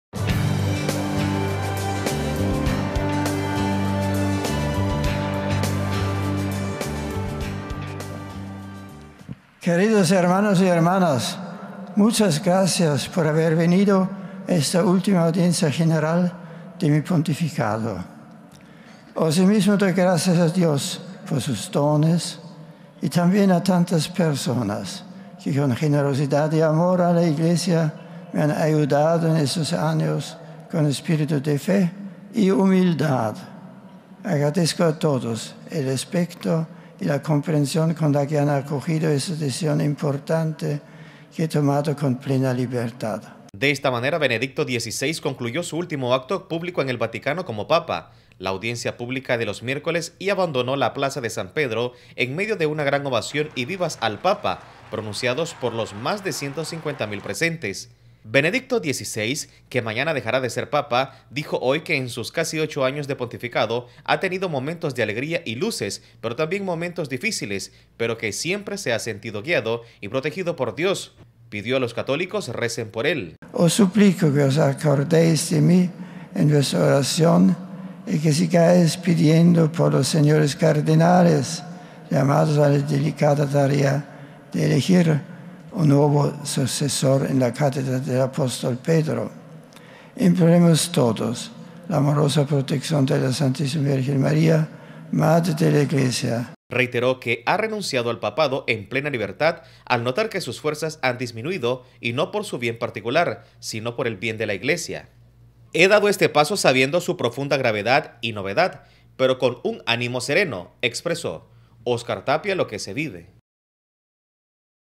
ULTIMA AUDIENCIA DE BENEDICTO XVI: "NO ESTOY SOLO"
EL PAPA CELEBREO EN EL VATICANO SU ULTIMA AUDIENCIA ANTE UNAS 150 MIL PERSONAS.